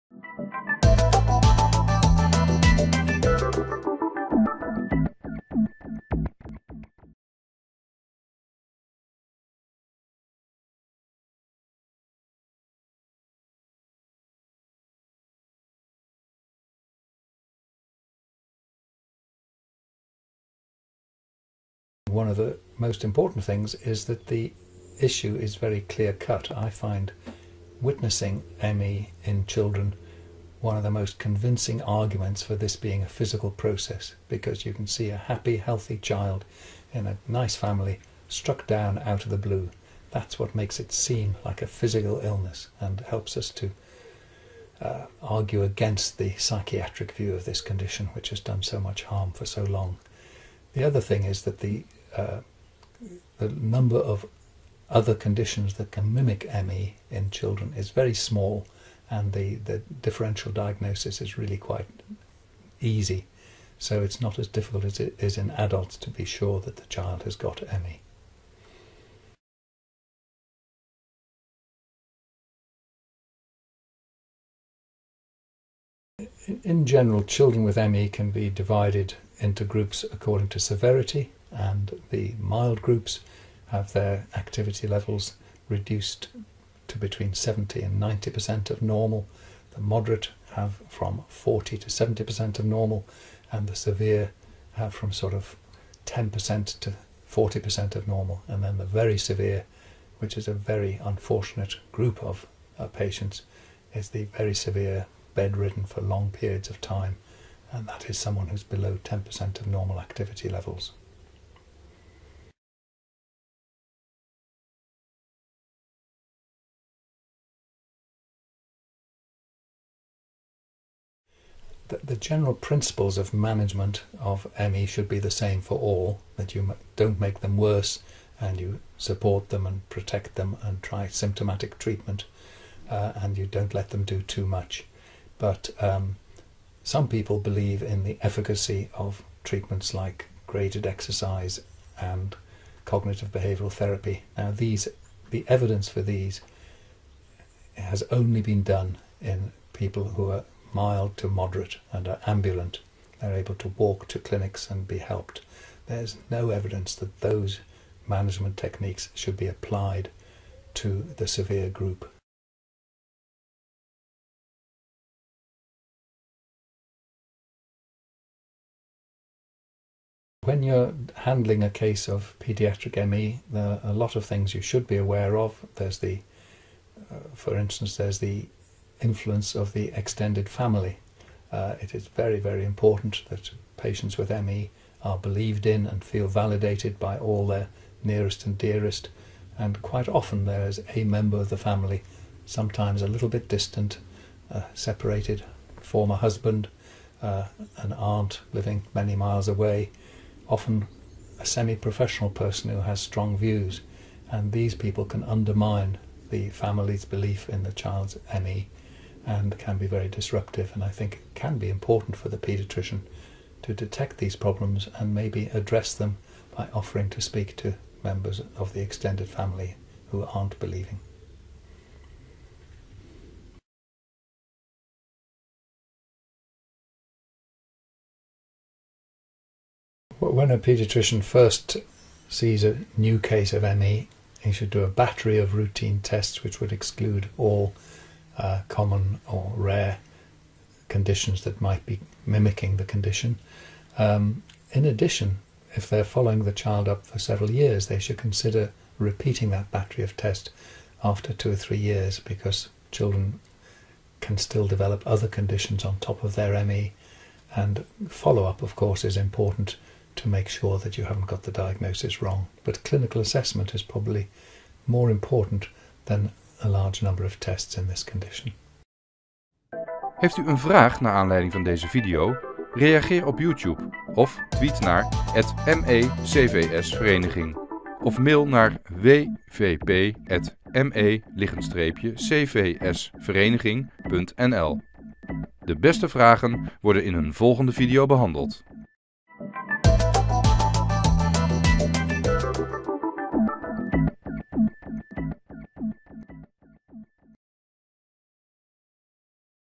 32. ME & children, part 1 / ME & kinderen, deel 1 - Dr. Nigel Speight (kinderarts)
(For English description, see below)

In dit college geeft Dr. Nigel Speight (kinderarts) antwoord op vragen over ME/cvs bij kinderen.

0.14 Wat zijn de hoofdkenmerken van ME bij kinderen?
1.10 Zijn er subgroepen kinderen met ME?
1.53 Zijn er voor diverse groepen diverse behandelingen?
2.43 Waar zouden kinderartsen op moeten letten?
3.43 Welke testen moeten kinderartsen zeker laten doen?

English:
In this web seminar Dr. Nigel Speight answers questions about ME/cfs in children.

0.14 What are the main characteristics of ME in children?
1.10 Are there subgroups of children with ME?
1.53 Are there different treatments for different subgroups?
2.43 What should paediatricians be aware of?
3.43 Which test should paediatricians prescribe?